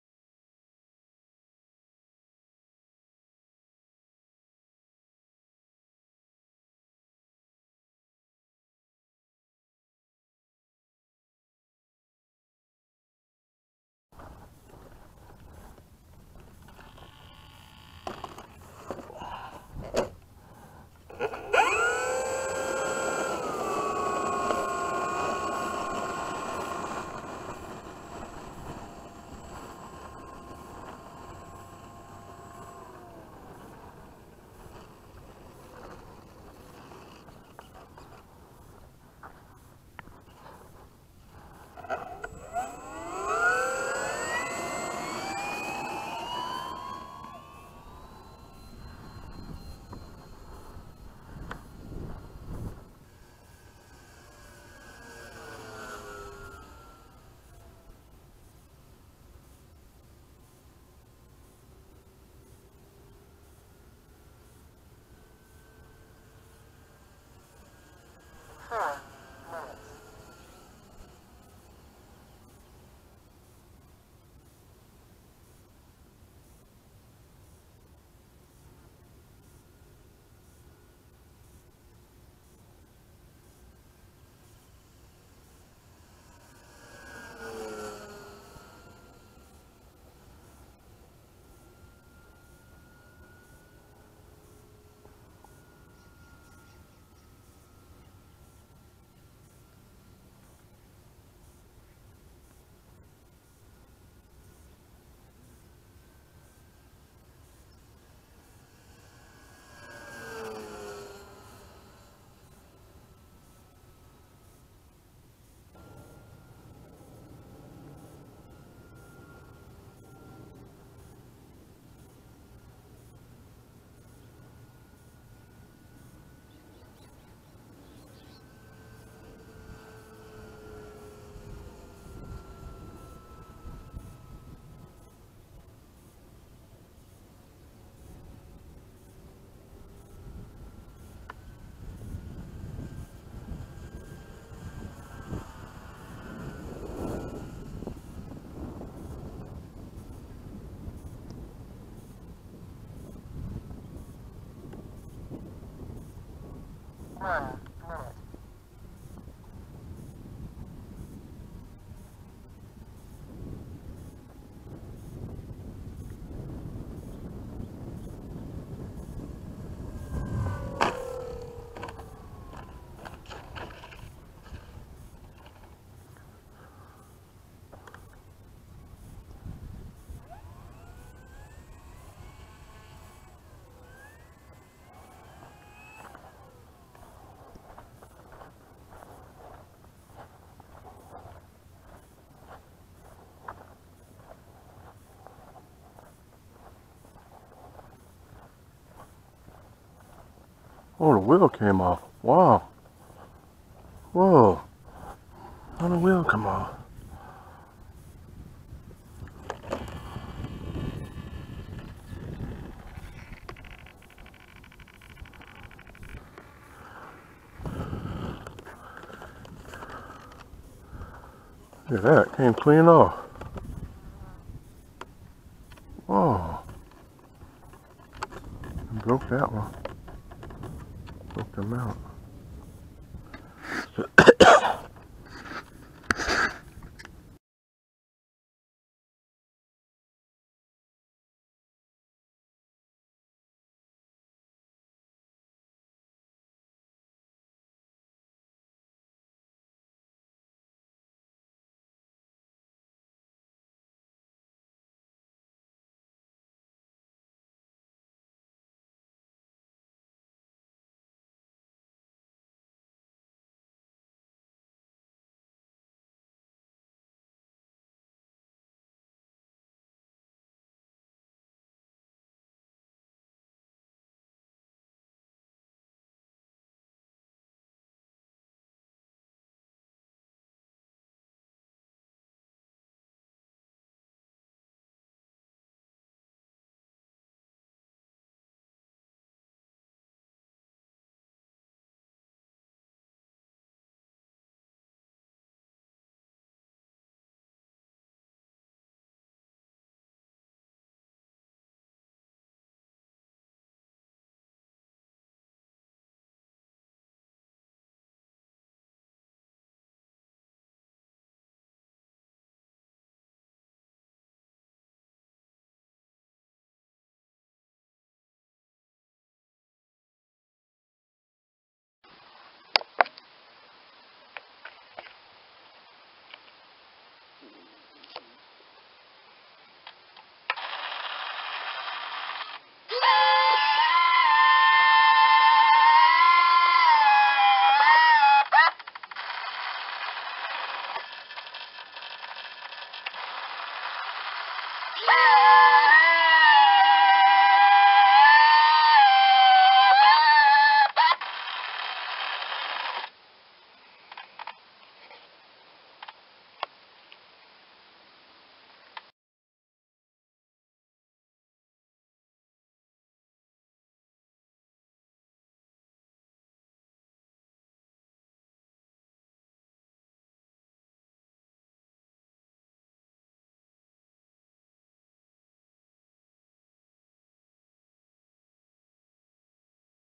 CVF Activity 2017 - Ferocious Frankie Retracts Fix
This video captures me flying my FMS P-51D Mustang and the subsequent failure of one of the retracts. The new retracts cost a lot and so I was able to upgrade for almost half the price.